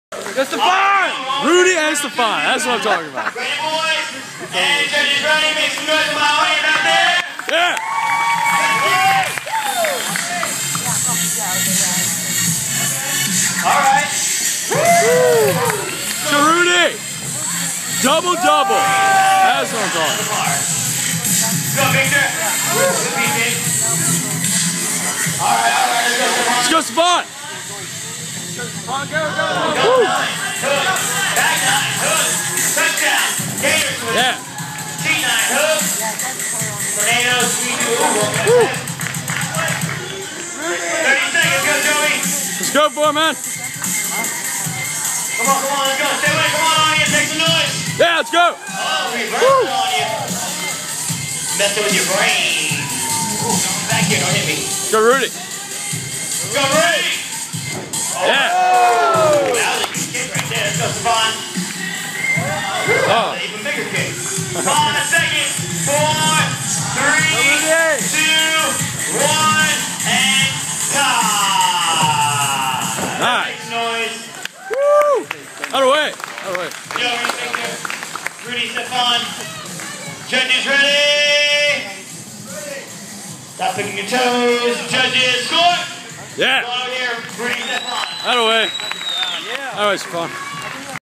Legacy Gathering 2012 - 2 on 2 Round 1
Rudy Meadows and I kickin' butt and takin names! Wahoooo